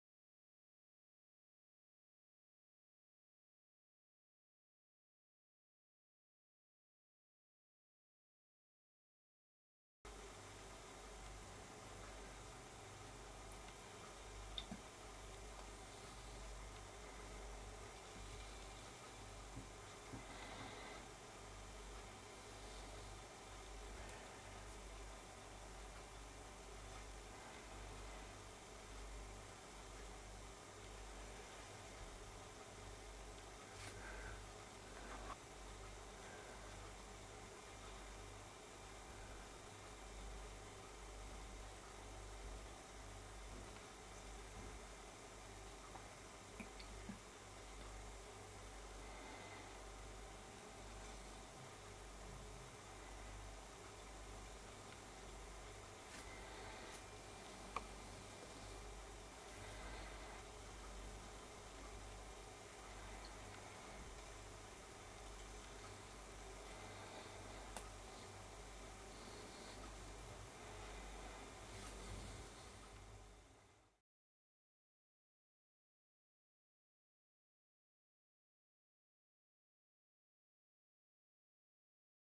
Houtduif in bad
Houtduif neemt een bad. Wood pigeon takes a bath. Amsterdam, Laplacestraat voorjaar 2010.